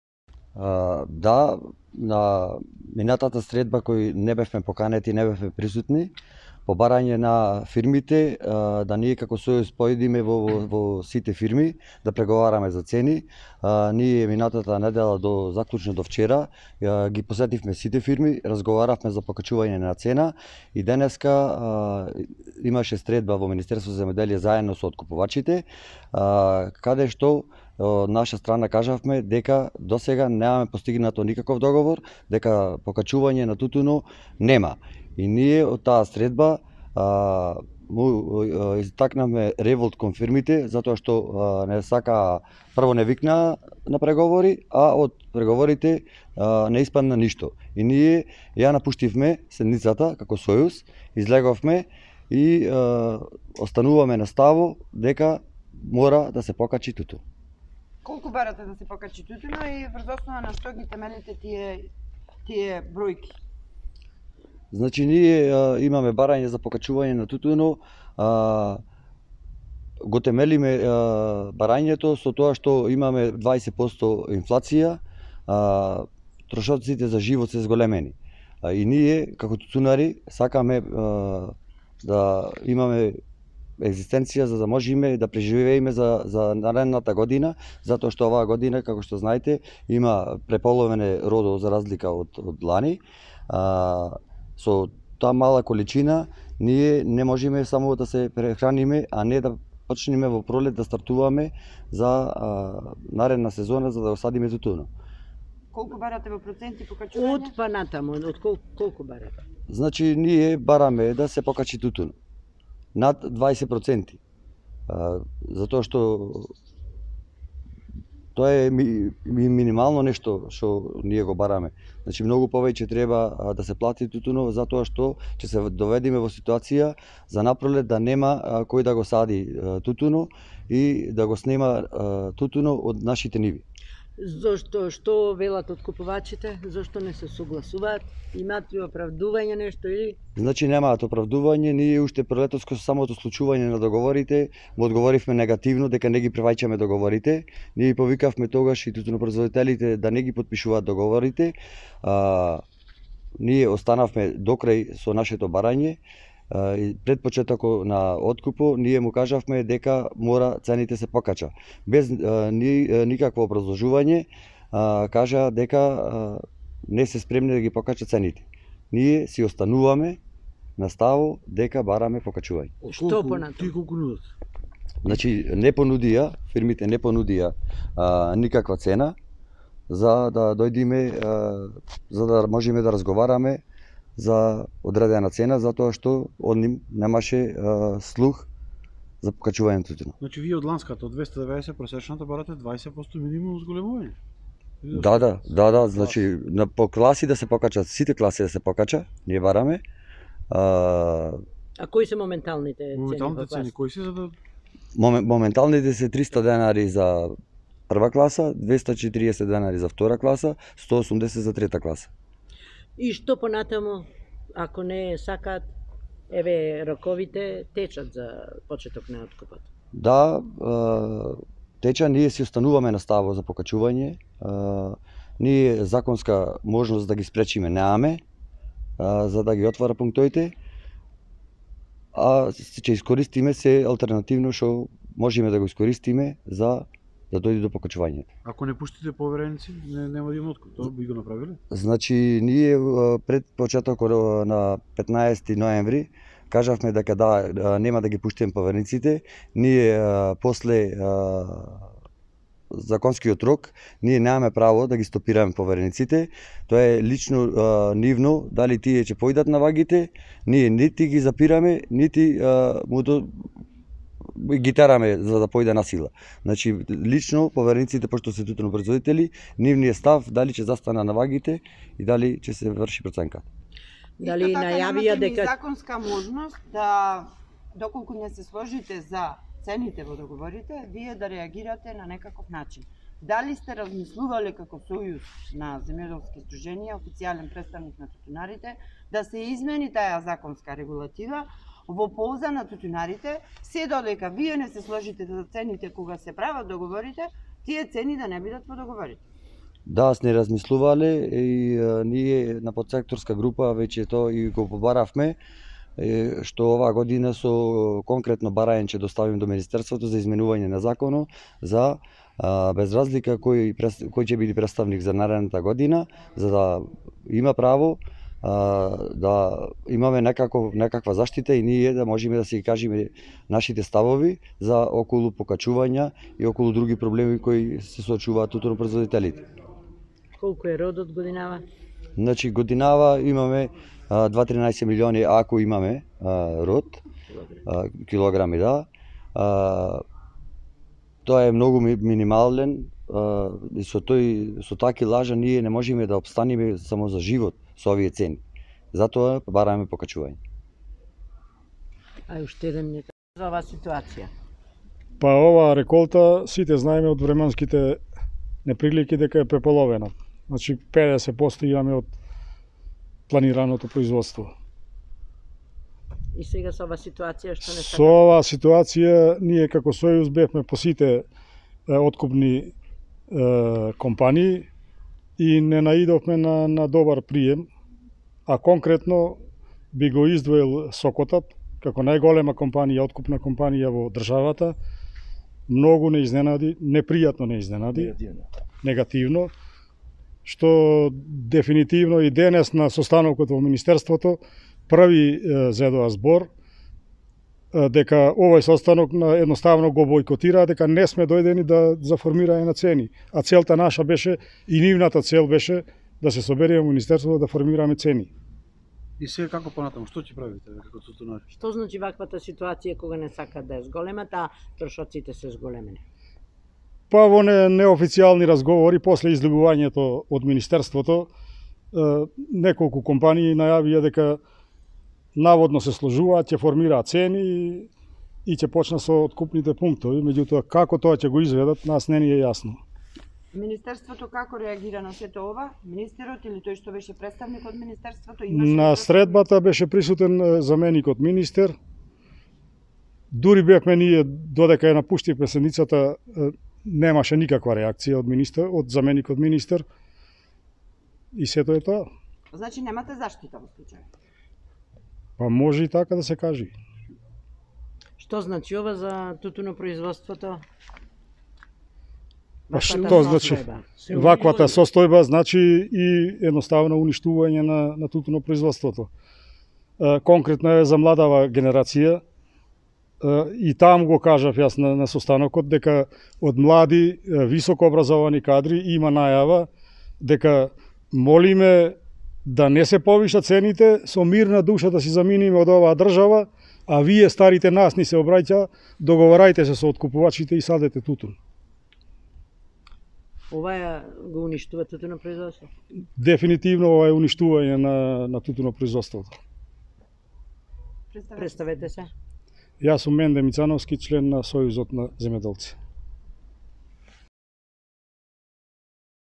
Нема договор за повисоки откупни цени на тутунот
Прес конференција на Сојузот на земјоделски здруженија